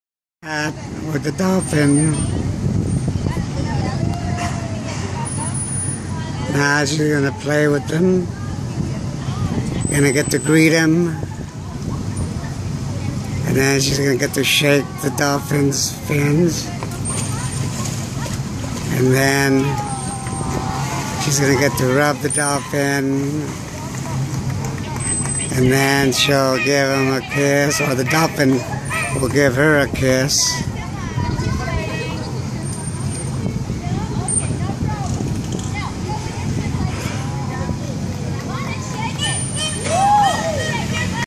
My friend is doing the swim with the Dolphins. I did this several years ago in the same place here called Vallarta Adventures. We are still enjoying my week stay at The Mayan Palace here in Nueva Vallarta..I will put up several other videos of some of the most interesting places and things we did.